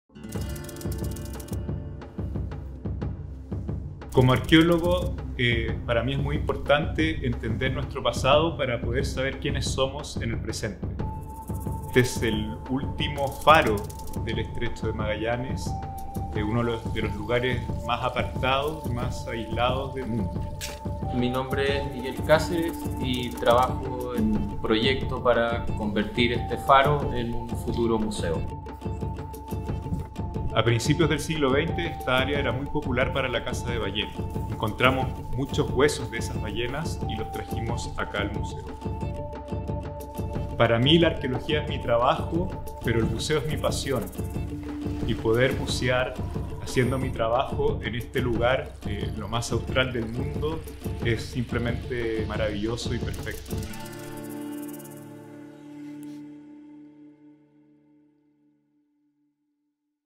Fay Archive - Field Test No. 16
The old San Isidro Lighthouse serves as the backdrop to our last stop on our Chilean adventure. A journey along this over 4000 km-long country with ever-differing landscapes, joined along the way by authentic yet fascinating individuals: Field Testers in the truest sense of the word, who put our jackets to the test as they provide us with a one-off glimpse of their unique country.